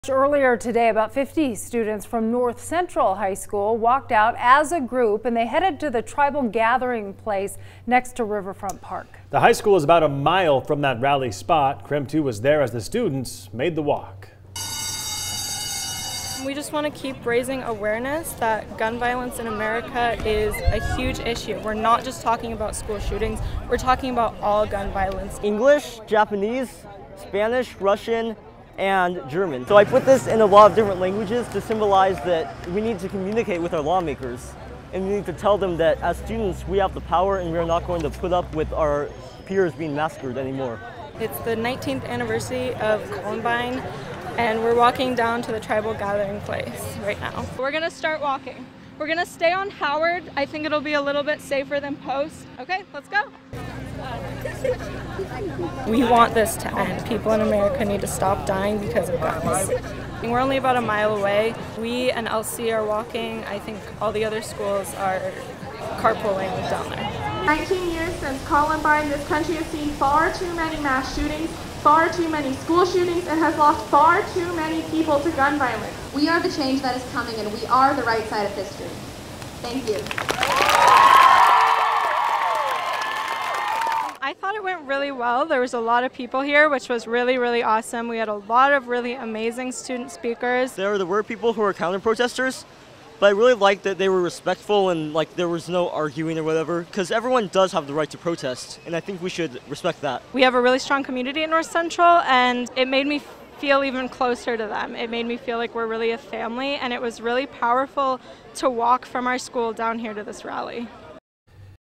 North Central High School students walk out of class
KREM 2 was there as the students made the walk.